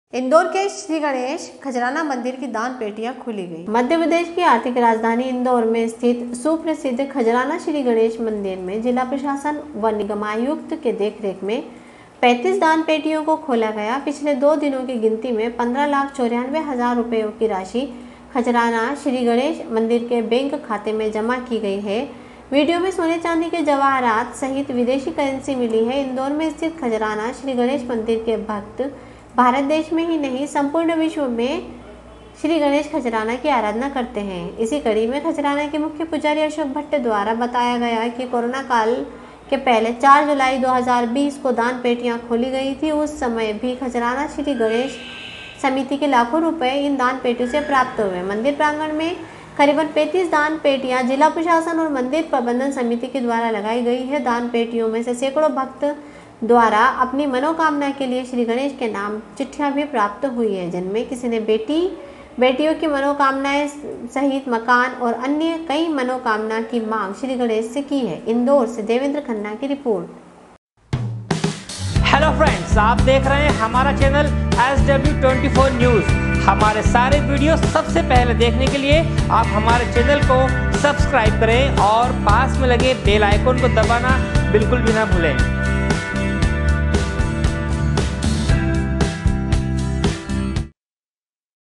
Indore- इंदौर में स्थित सुप्रसिद्ध खजराना श्री गणेश मन्दिर में जिला प्रशासन व निगमायुक्त के देख रेख
मध्य प्रदेश की आर्थिक राजधानी इंदौर में स्थित सुप्रसिद्ध खजराना श्री गणेश मन्दिर में जिला प्रशासन व निगमायुक्त के देख रेख में 35 दान पेटियों को खोल गया। इंदौर से देवेंद्र खन्ना की रिपोर्ट